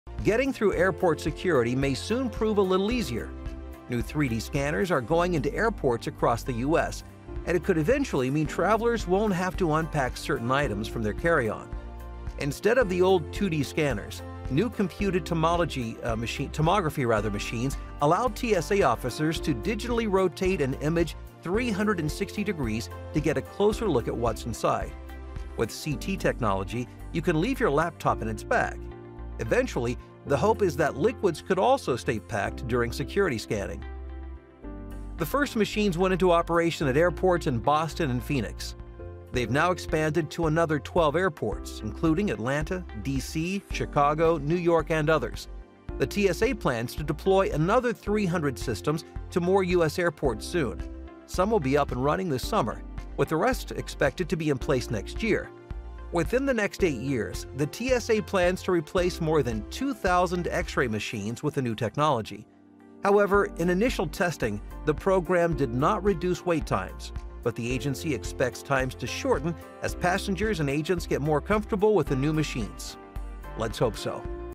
3D Scanning Tech Keeps Electronics & Liquids in Bags
Getting through airport security may soon prove a little easier. New 3-D scanners are going into airports across the US.